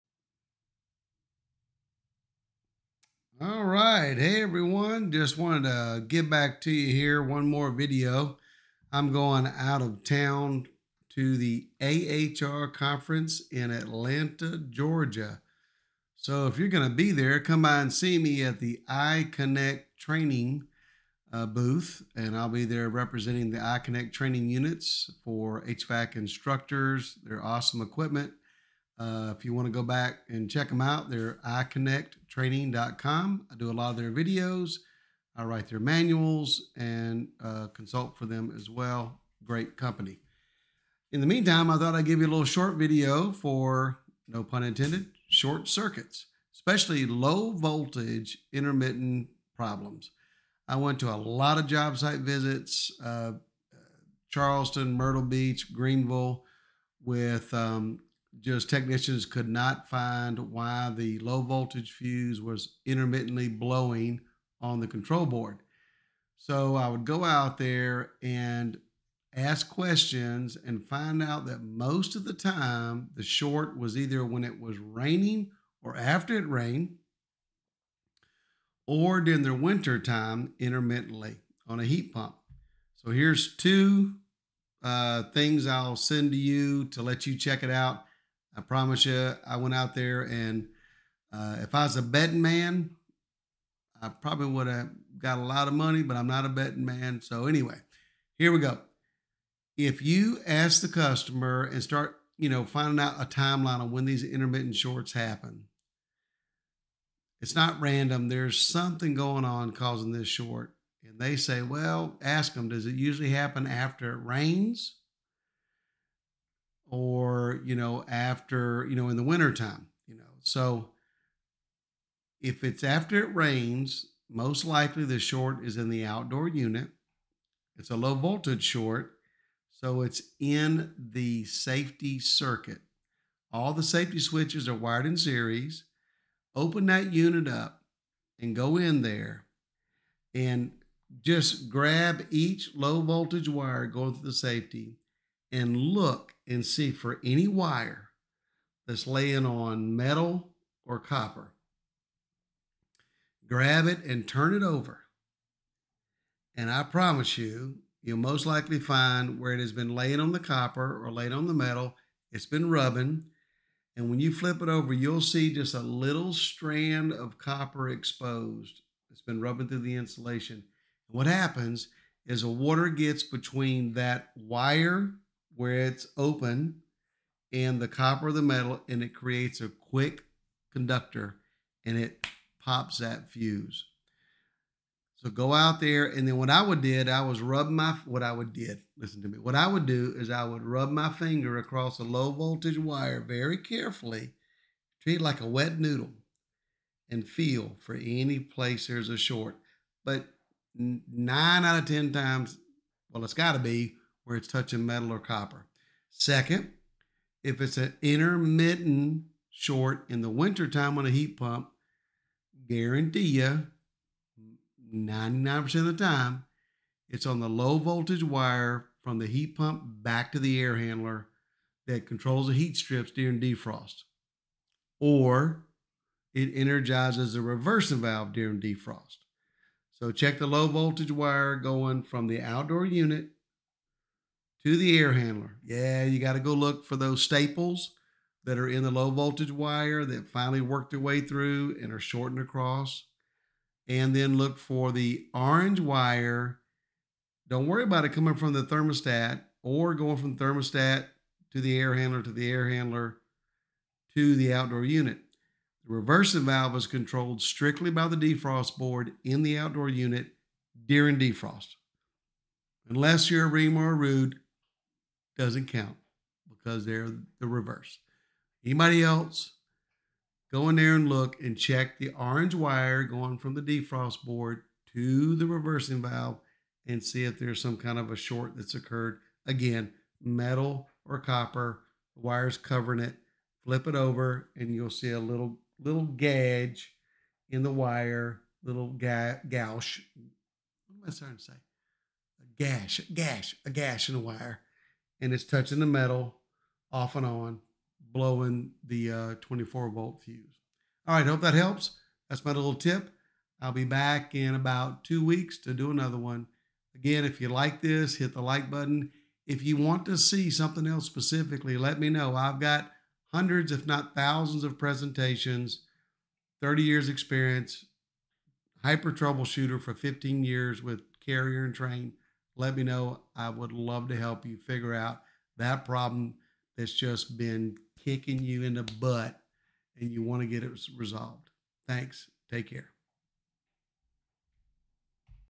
Low Voltage Shorts--simplified!
A short video on the common low voltage shorts and how to find them.  Also, I am going to AHR in Atlanta, Ga.  Come see me at the iConnect training booth!  In this video, I'll show you how to identify and fix low voltage shorts using a simple test.  Low voltage shorts can be a PAIN if not fixed quickly, so be sure to watch this video and learn how to fix low voltage shorts quickly and safely!